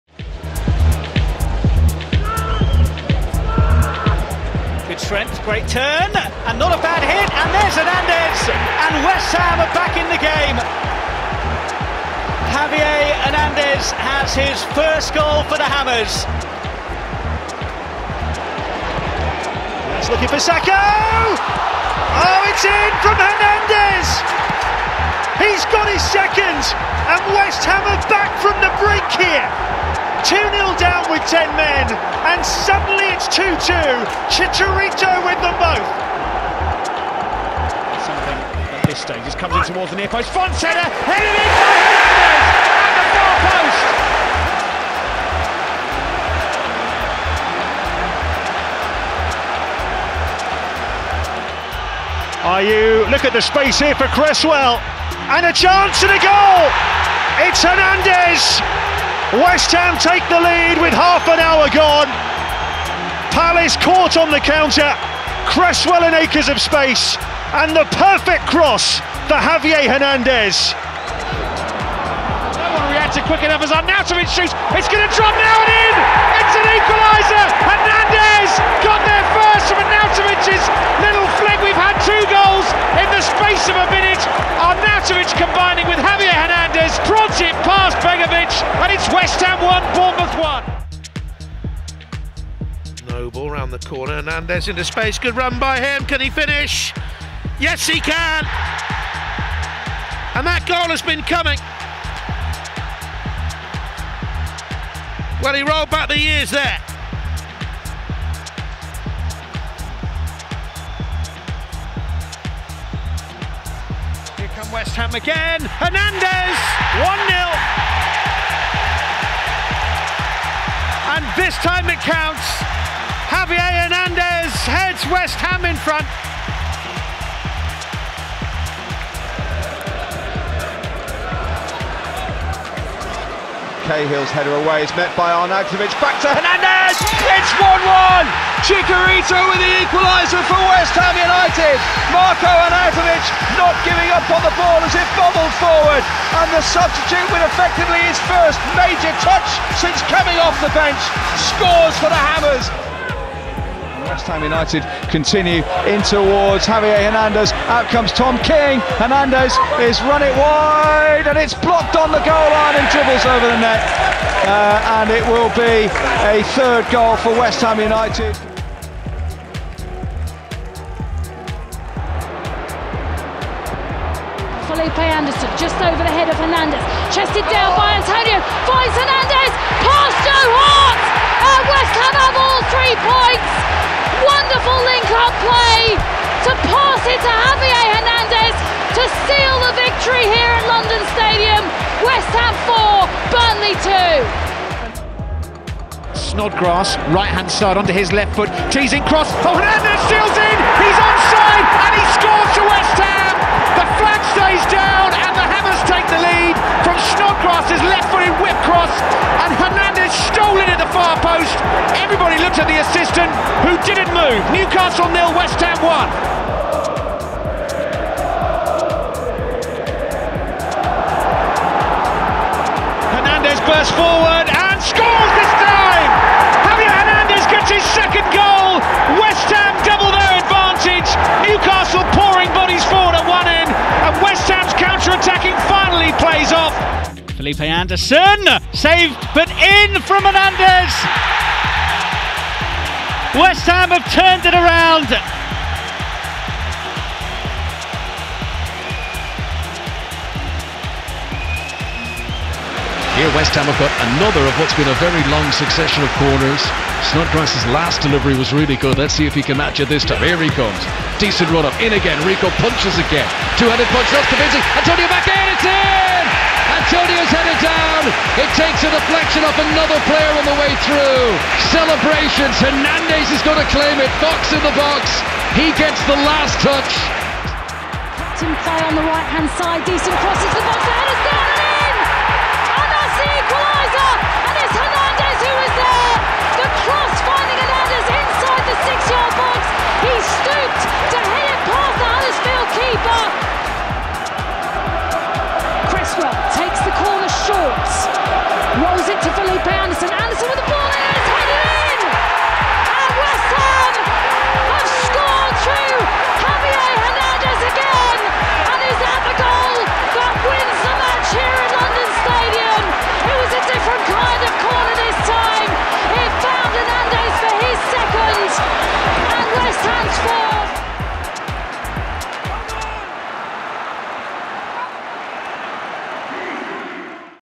EVERY CHICHARITO GOAL FOR WEST HAM
It's two years today since we signed Chicharito! What's your favourite Chicha goal for West Ham?